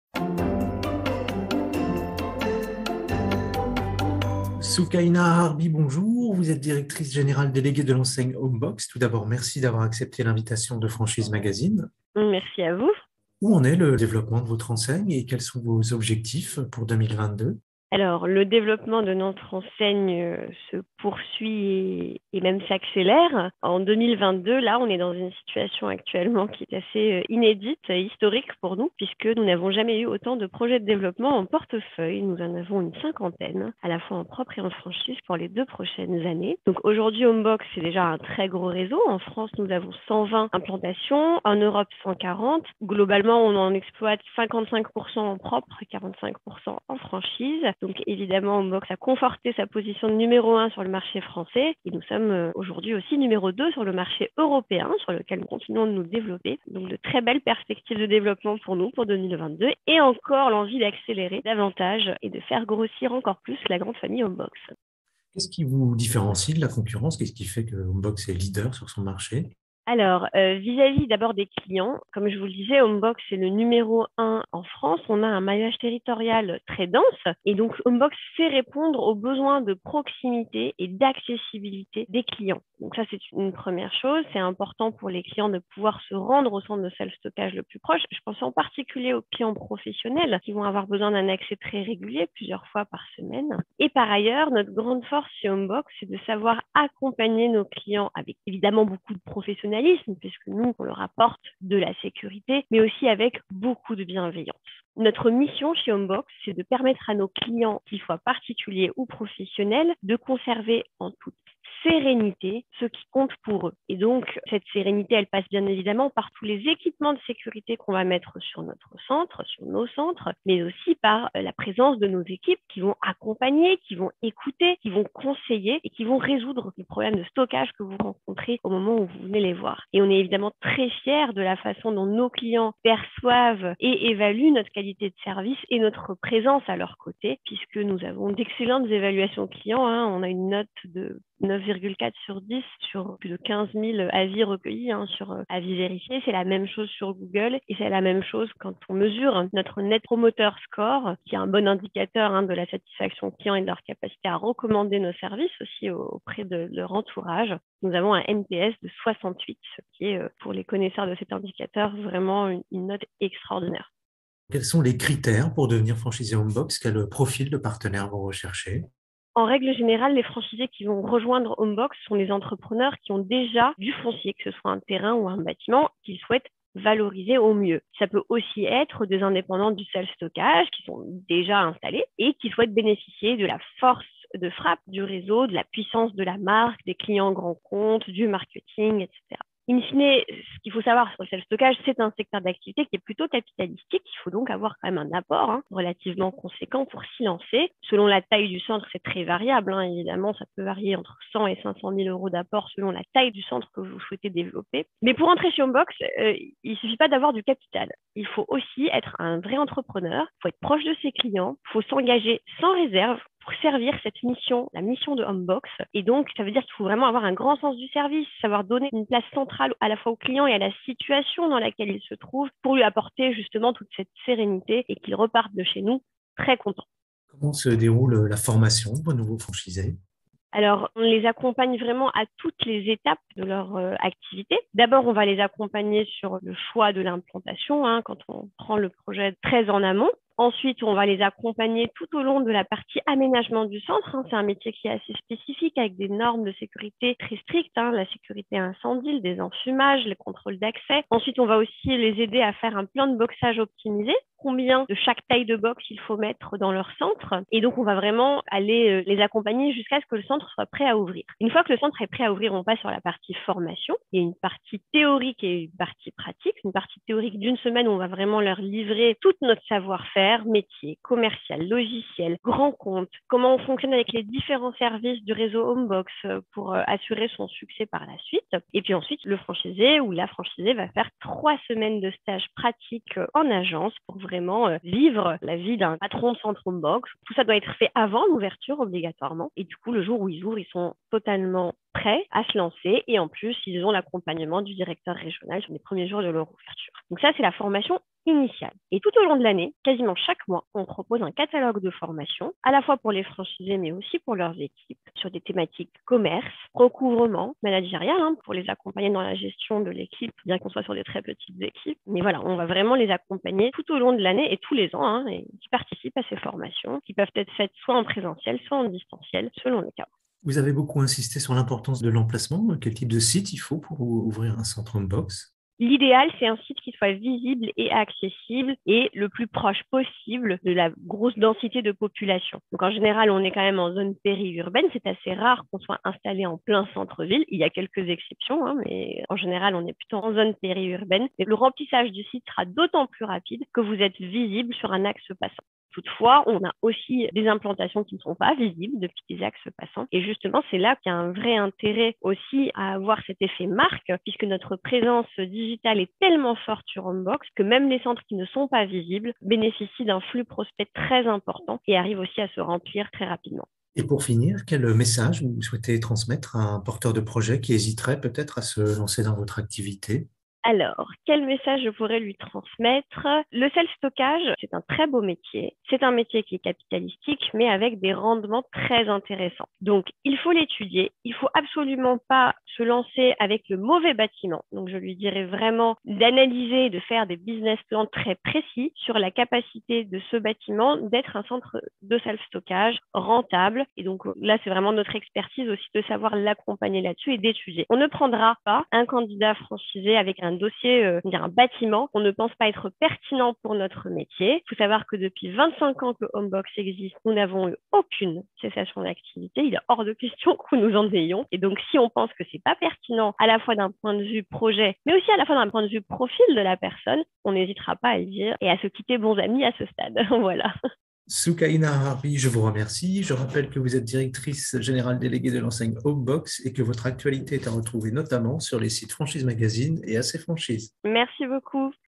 Interview Soukaïna Arharbi Directrice générale déléguée de Homebox - Franchise Magazine
Soukaïna Arharbi, Directrice générale déléguée de l’enseigne spécialiste du self-stockage Homebox, nous explique les grands axes de sa stratégie d’expansion.